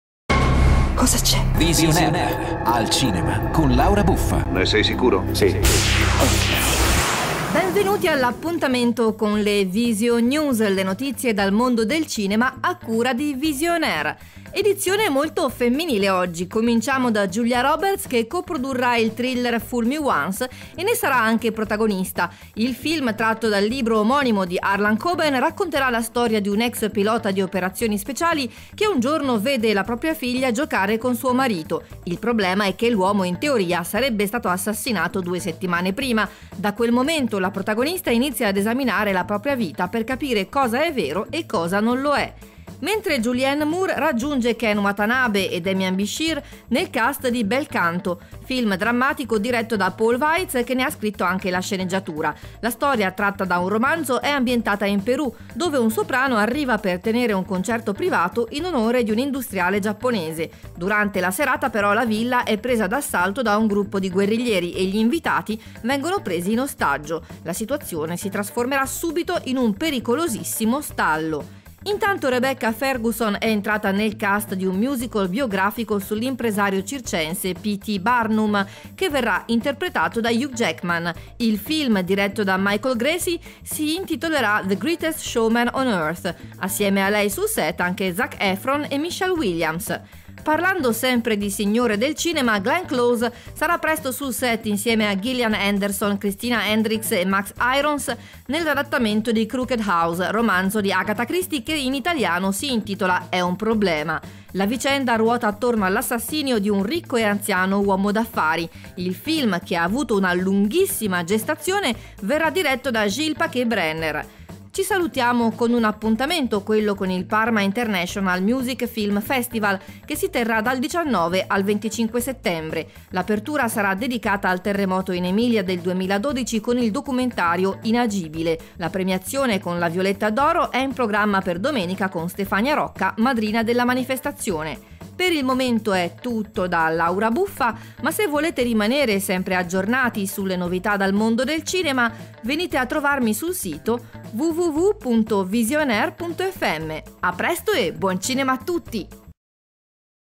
Il mistero di Julia Roberts
Julianne Moore ostaggio in Bel Canto.
Rebecca Ferguson al circo.
Glenn Close e Agatha Christie.
Parma International Music Film Festival.